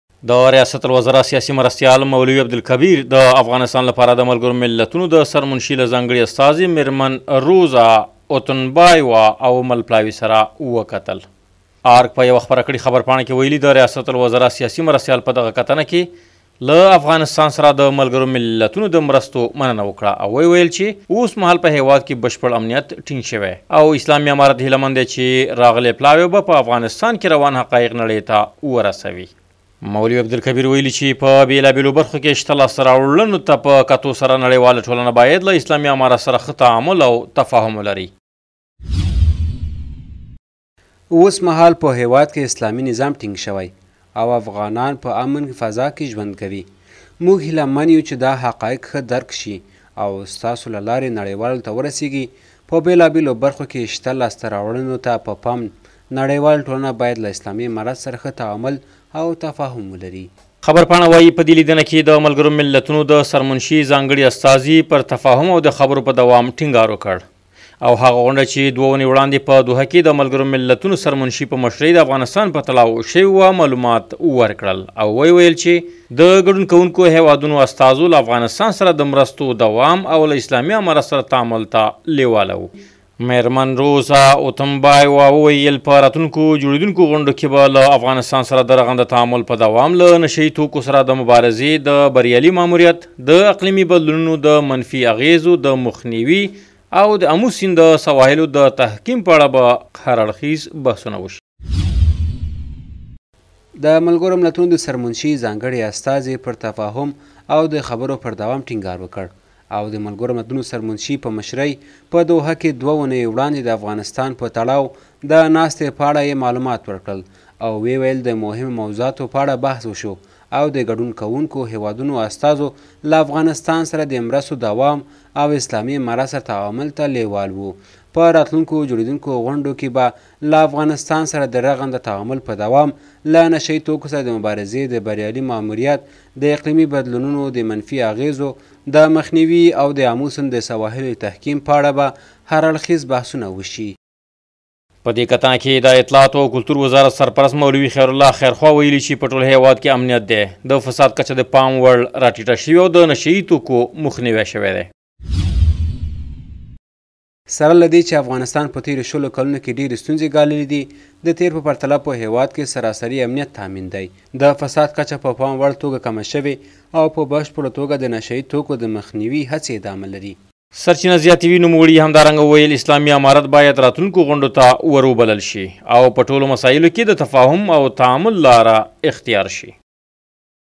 د ریاست الوزرا سیاسي مرستیال مولوي عبدالکبیر د ملګرو ملتونو د سرمنشي له ځانګړې استازې مېرمن روزا
د ریاست الوزرا سیاسي مرستیال مولوي عبدالکبیر د افغانستان لپاره د ملګرو ملتونو د سرمنشي له ځانګړې استازې مېرمن روزا اوتونبایوا او مل پلاوي سره وکتل.
ارګ وایي مولوي عبدالکبیر نوموړې ته وویل چې اوسمهال په هېواد کې بشپړ امنيت ټينګ شوی او اسلامي امارت هيله من دی، چې راغلی پلاوی به په افغانستان کې روان حقايق نړۍ ته ورسوي.
مولوي عبدالکبیر ویلي چې په بېلابېلو برخو کې شته لاسته راوړنو ته په کتو سره نړیواله ټولنه باید له اسلامي امارت سره ښه تعامل او تفاهم ولري.
مېرمن روزا اوتونبایوا بیا وایي چې په راتلونکو جوړېدونکو غونډو کې به له افغانستان سره د رغنده تعامل په دوام، له نشه یي توکو سره د مبارزې د بریالي ماموریت، د اقلیمي بدلونونو د منفي اغېزو د مخنیوي او د اموسیند د سواحلو د تحکیم په اړه به هراړخیز بحثونه وشي.